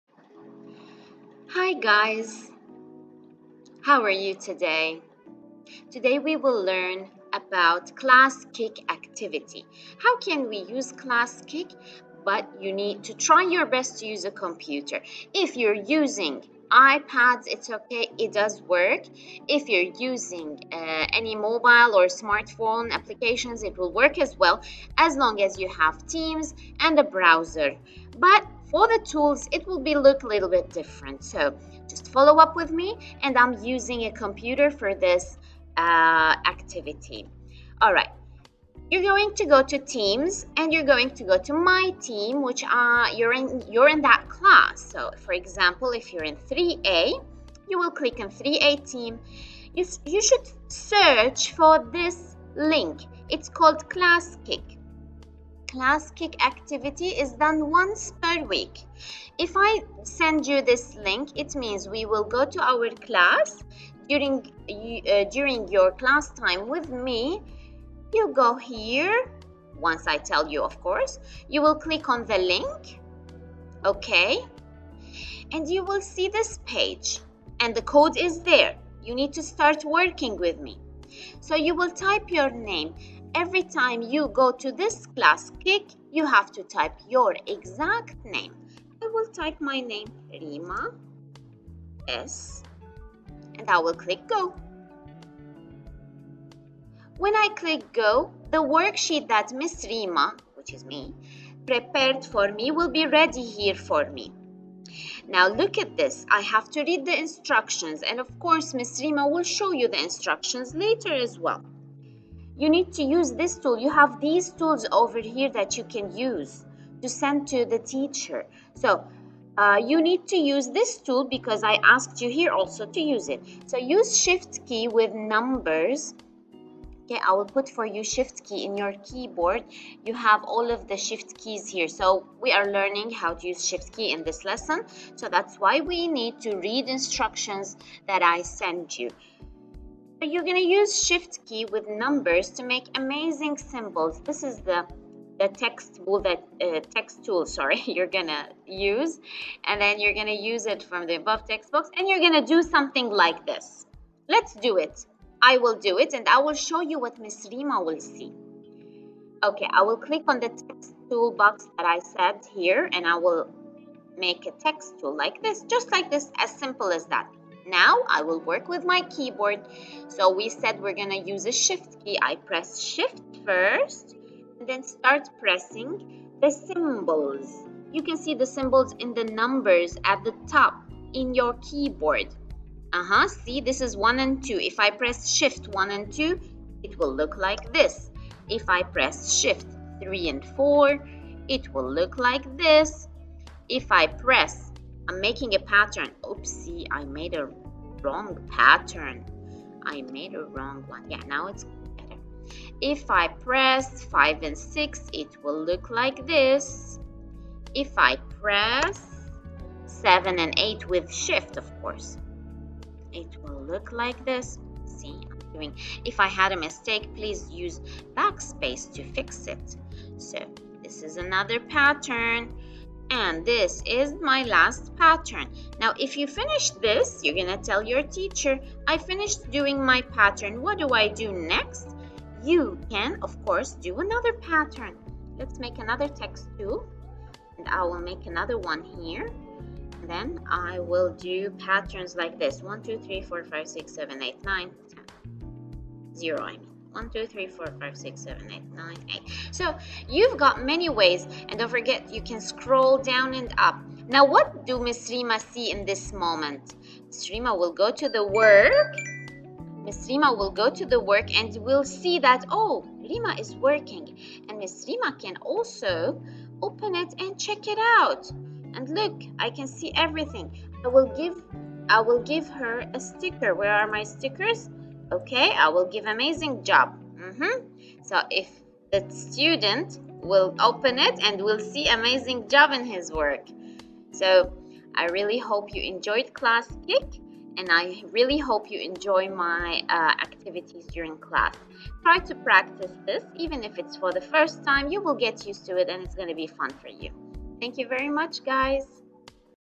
Using classkick during online activity
Your work saves alone automatically so don't worry, this is a live classwork.